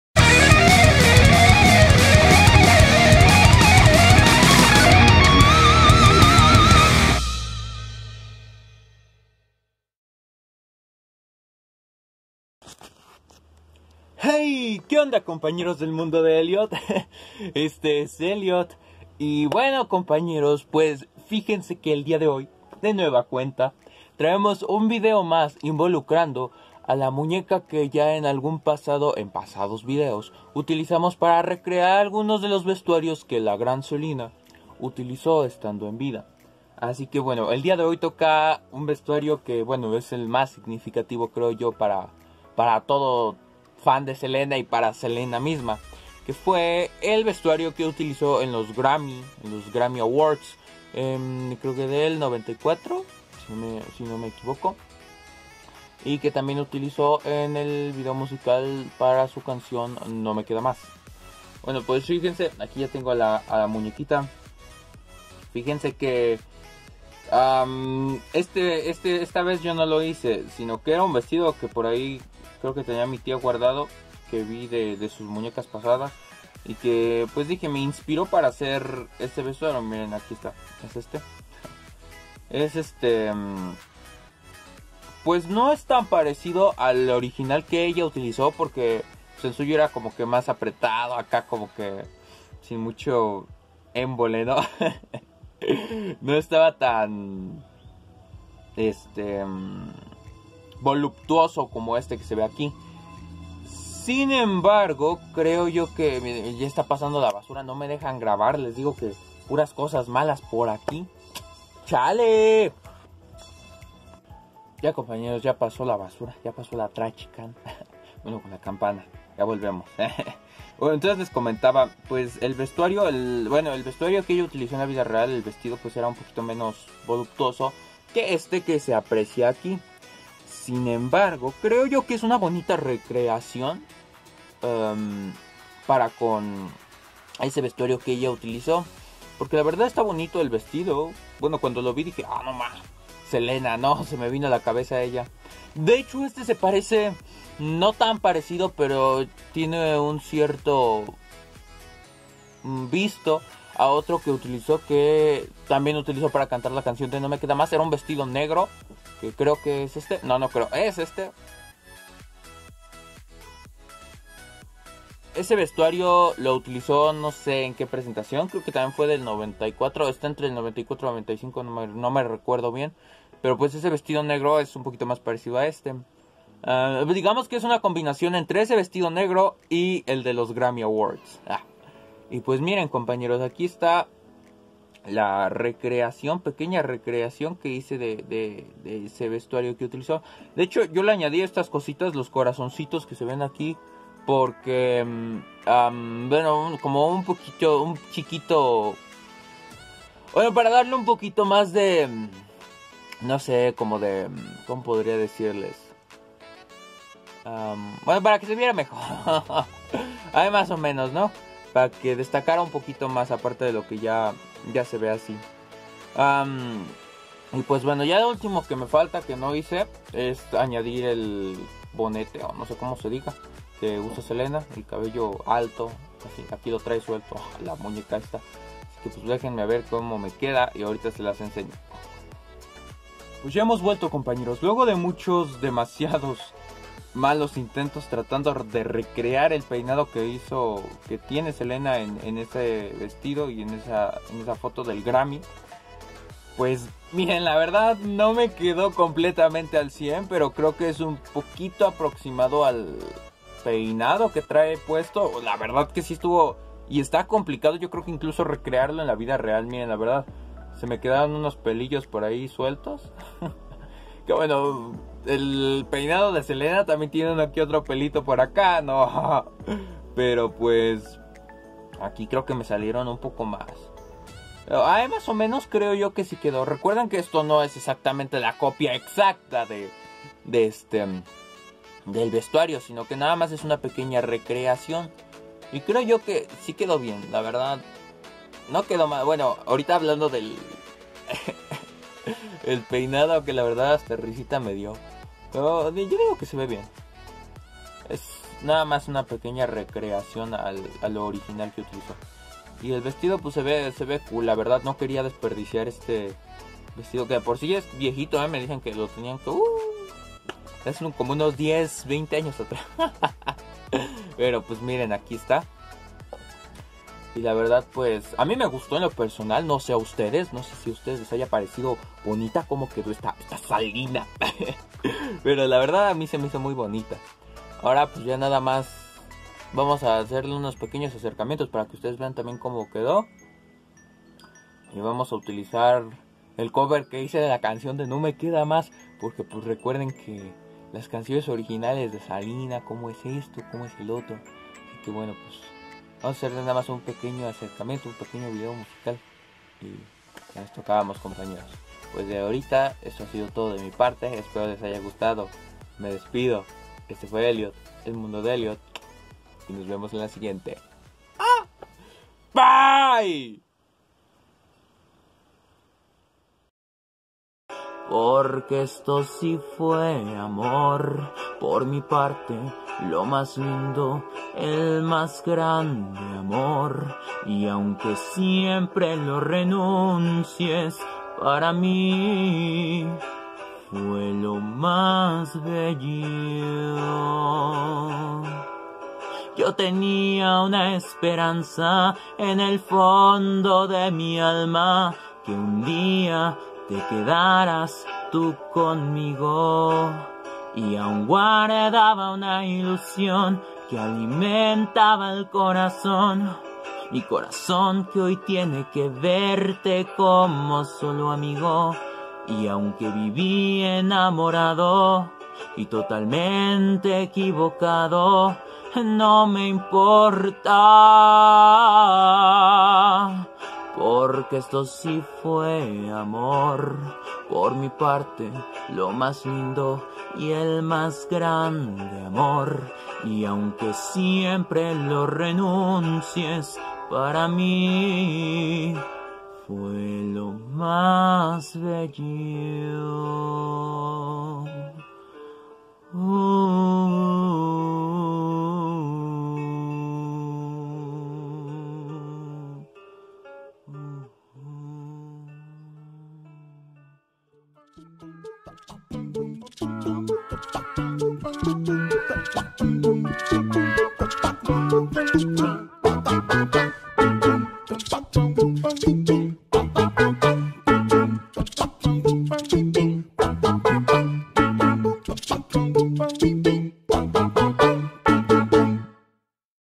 Muñeca de Selena Quintanilla 4
perdónenme la tardanza pero he tenido mucha tarea jsjsjs, sin embargo aquí nos encontramos de nueva cuenta, el día de hoy realice este pequeño tributo para Salina uwu, espero les guste, un saludo y un abrazo muy grande para todos jj ;v